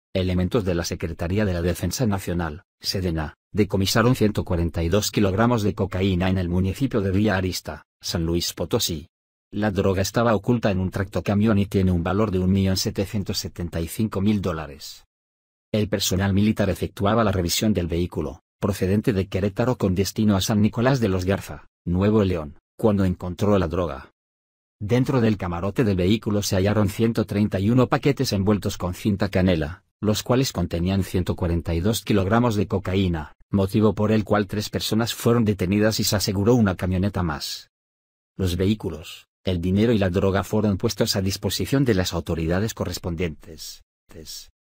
Hallan droga con valor de más de 1.7 mdd en SLP
Elementos de la Secretaría de la Defensa Nacional (Sedena) decomisaron 142 kilogramos de cocaína en el municipio de Villa Arista, San Luis Potosí. La droga estaba oculta en un tractocamión y tiene un valor de un millón 775 mil dólares.

El personal militar efectuaba la revisión del vehículo, procedente de Querétaro con destino a San Nicolás de los Garza, Nuevo León, cuando encontró la droga.

Dentro del camarote del vehículo se hallaron 131 paquetes envueltos con cinta canela, los cuales contenían 142 kilogramos de cocaína, motivo por el cual tres personas fueron detenidas y se aseguró una camioneta más.

Los vehículos, el dinero y la droga fueron puestos a disposición de las autoridades correspondientes.